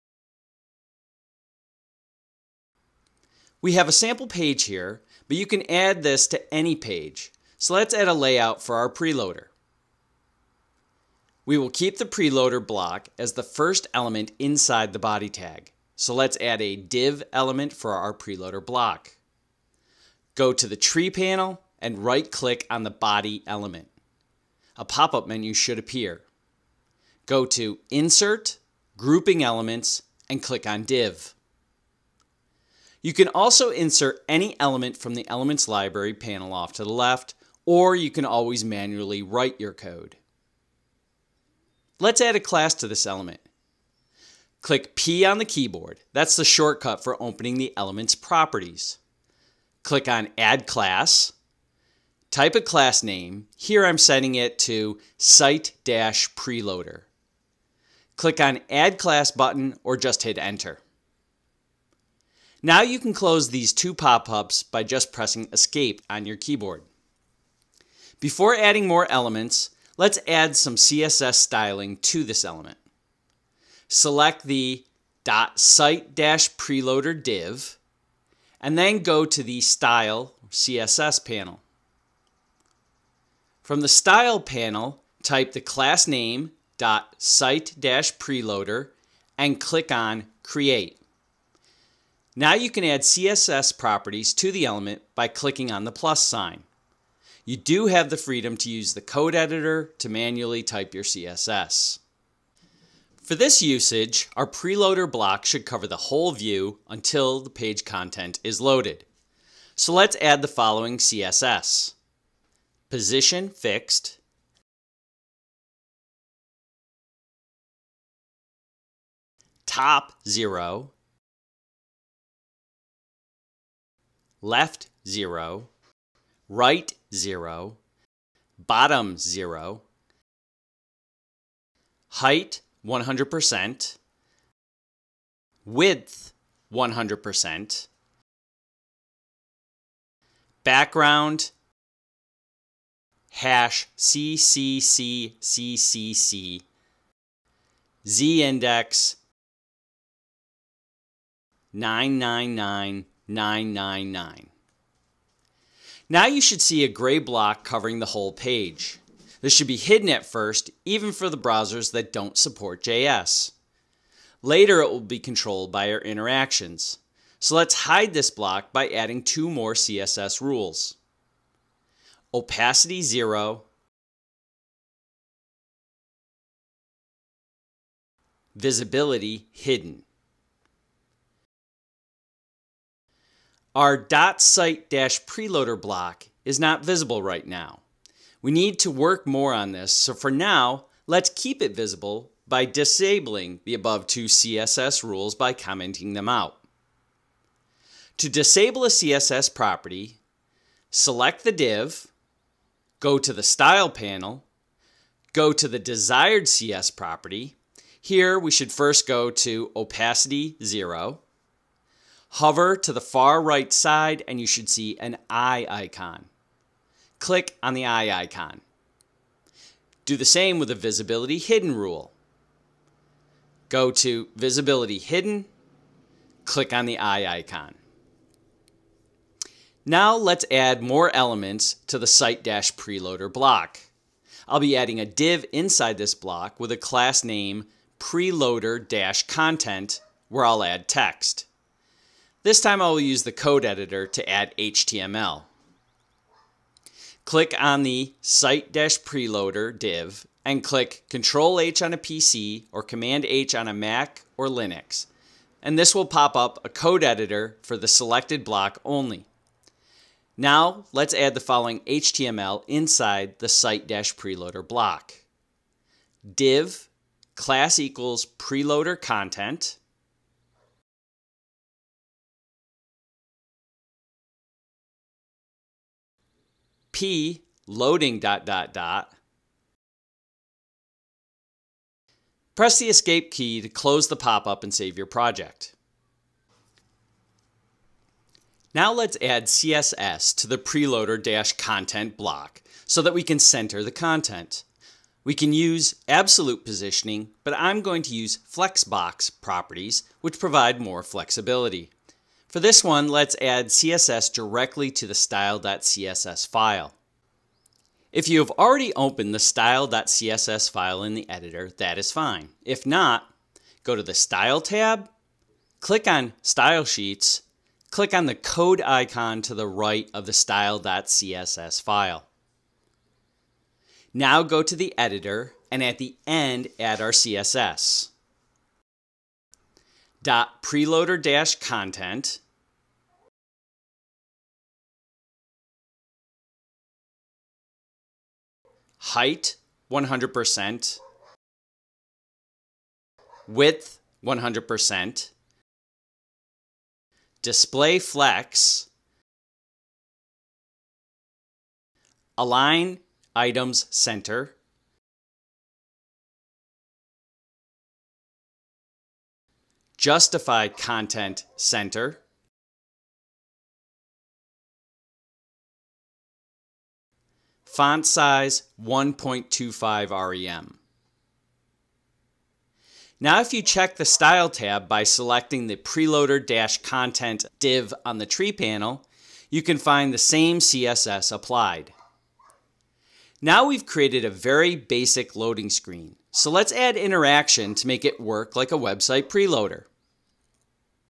2. HTML Layout and CSS Styling - Loading Screen Pinegrow Interactions Tutorial
A step-by-step tutorial about creating an animated loading screen with Pinegrow Interactions.